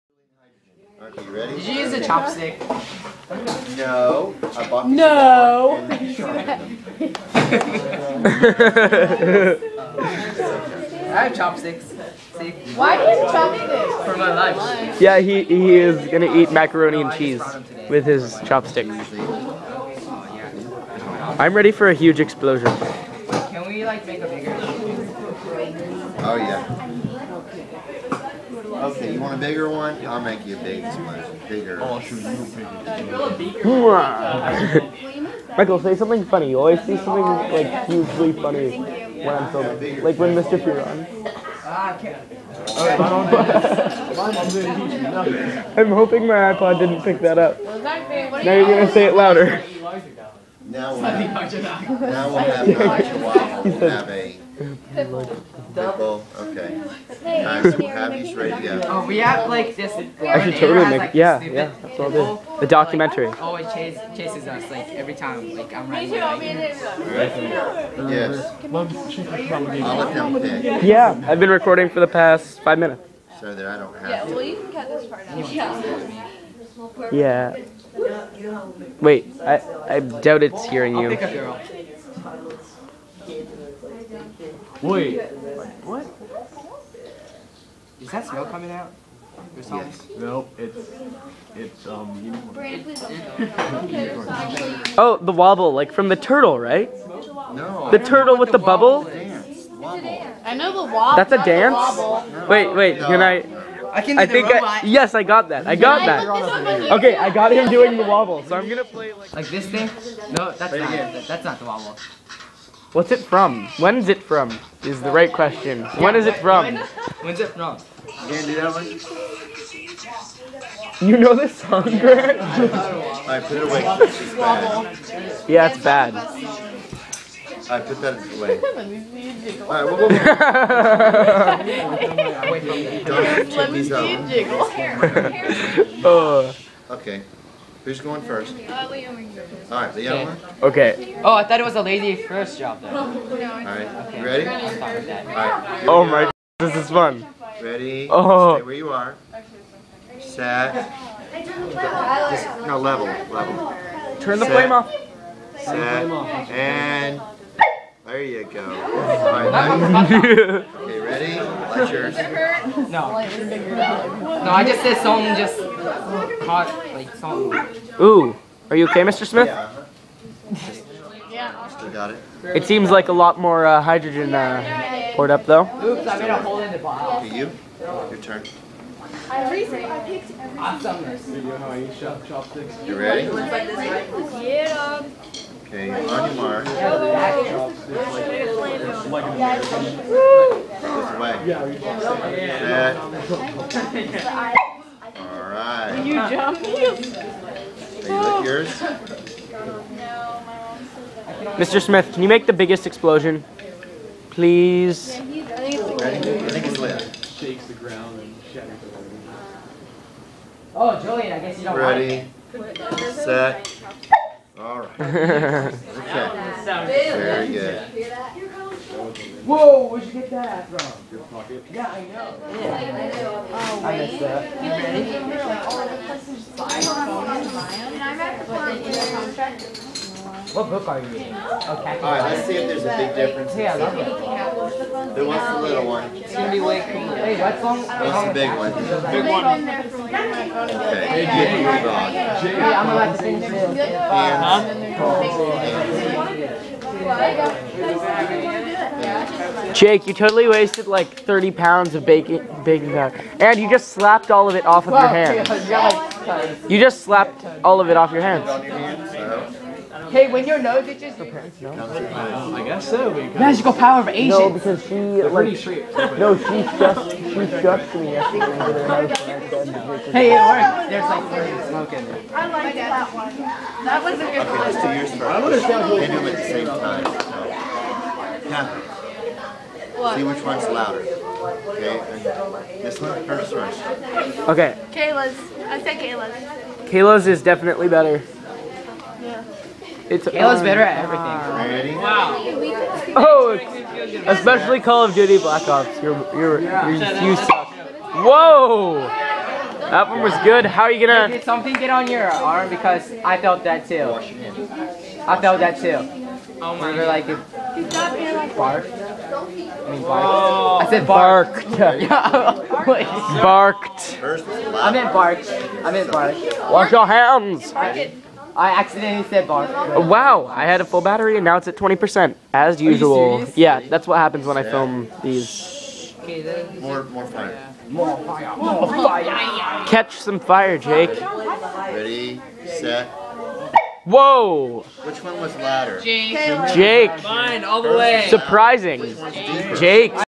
Mixing Acids and Bases
Mr. Smith's Third Period Class Blowing Up Hydrogen